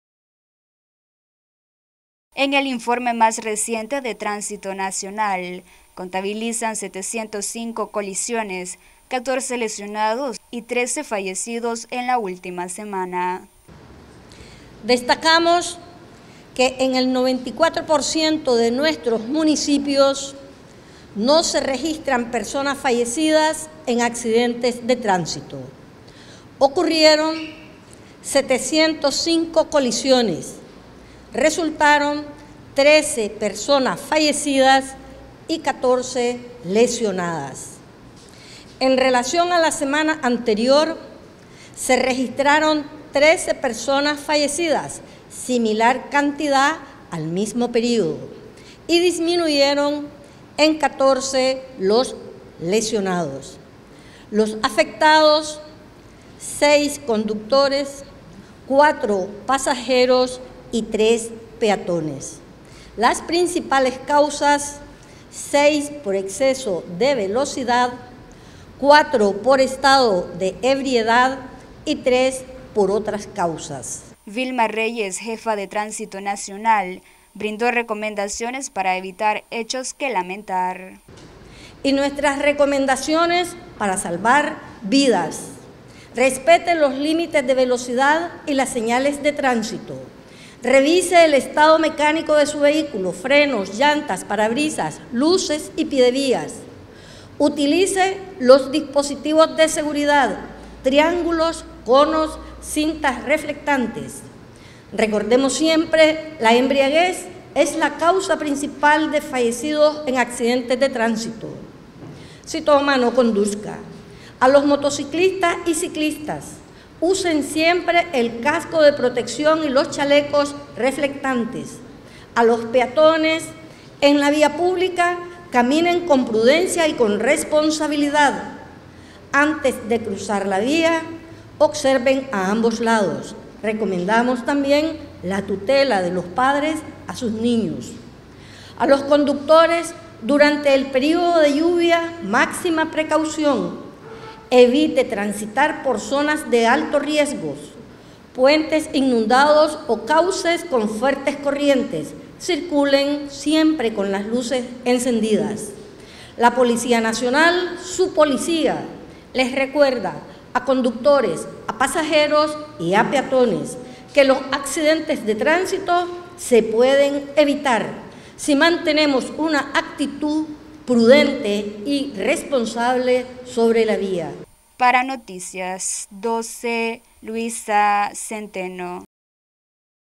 Tránsito nacional contabiliza 705 colisiones, 14 lesionados y 13 fallecidos en la última semana
Noticias12Nicaragua Tránsito nacional contabiliza 705 colisiones, 14 lesionados y 13 fallecidos en la última semana.